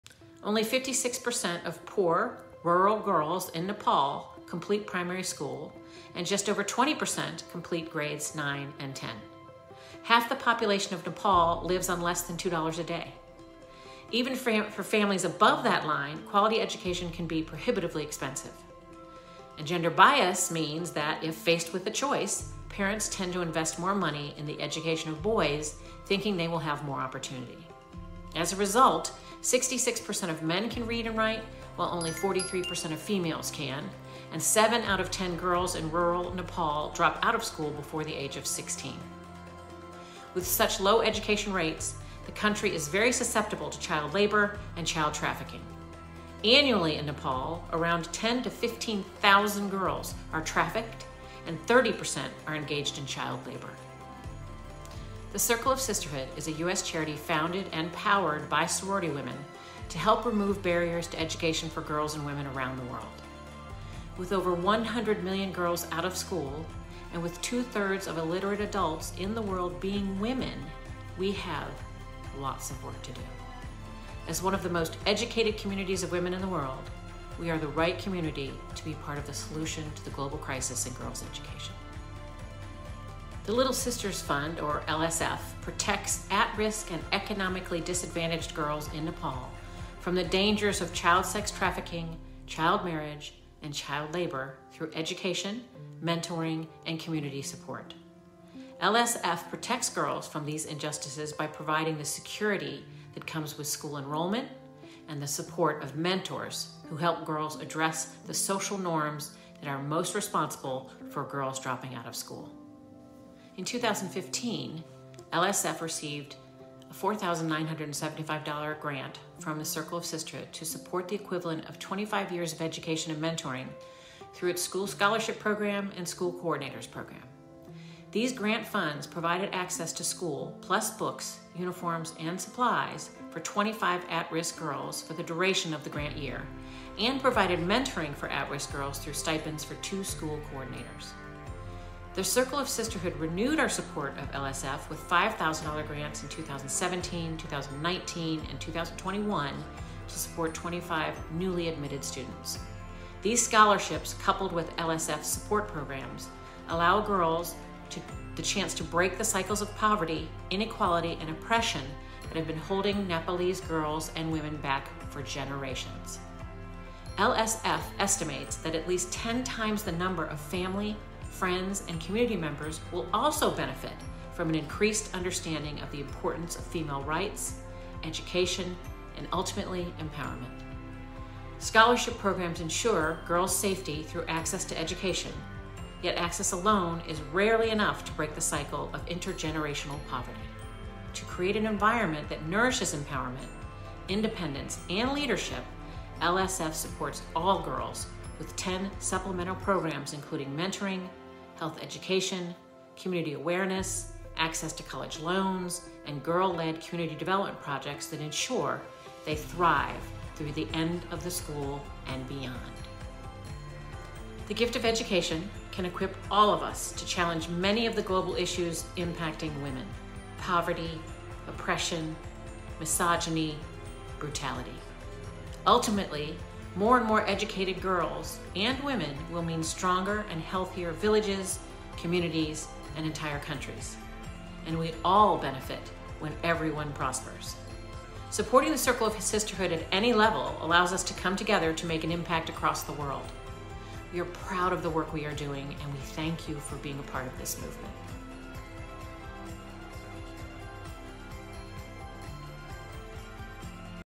How We Work to Remove Barriers to Education - Little Sisters Fund
Check out the latest episode of the monthly Circle of Sisterhood vlog series! CofS has awarded multiple funding grants to the Little Sisters Fund (LSF) where they are protecting at-risk girls in Nepal from the dangers of child sex trafficking, child marriage, and child labor through access to education. Take a moment to learn more about how our grants have helped to change the lives of so many girls through the work of LSF.
Volume 2, Episode 6
Education Barrier Topic: School Supplies/Infrastructure, Teacher Salaries/Education, K-12 Education
Location: Kathmandu, Nepal
Grant Years: 2023, 2021, 2019, 2017, 2015